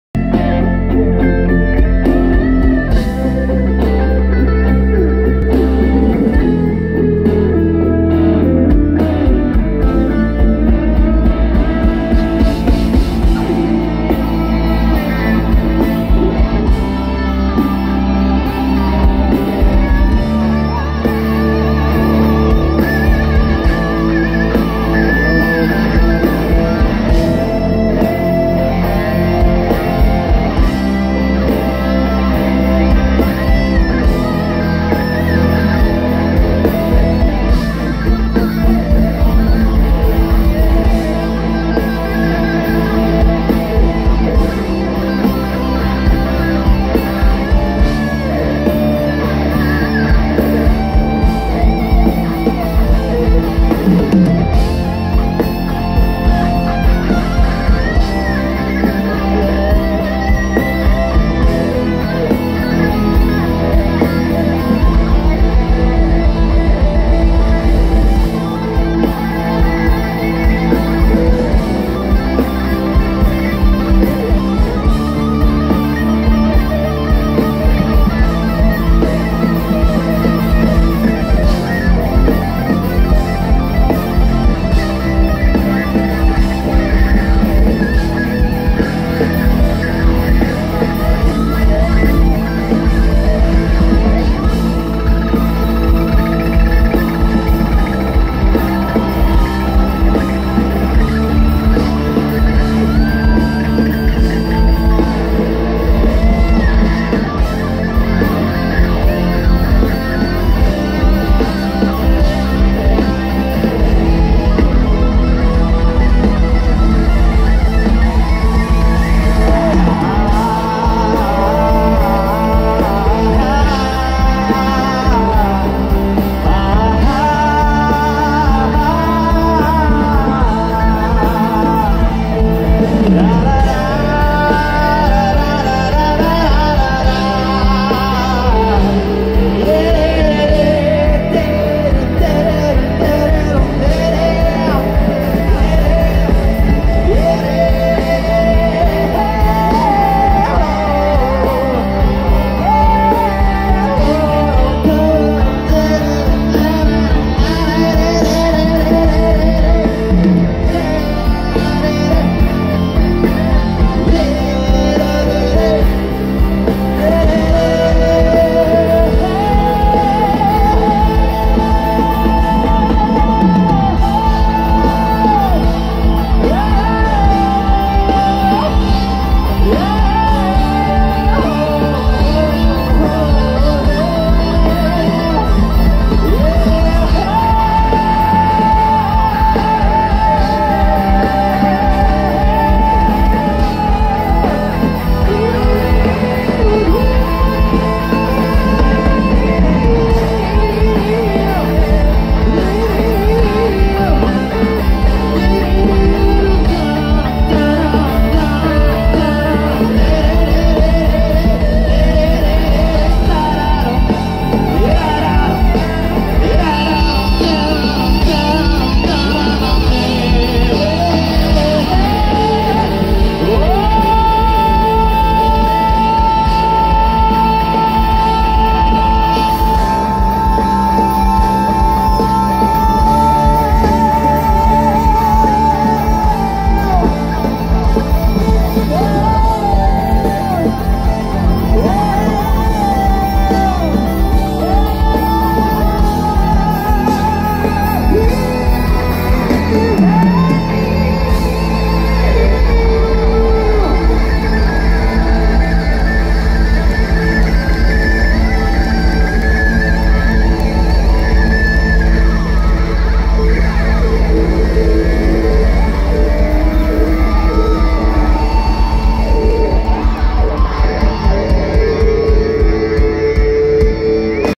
Thaikkudam Bridge - Guitar Solo (Live)
Guitar solo from Thaikkudam bridge lead guitarist Mithun Raju, LIVE at AFMC pune on May 7th 2022.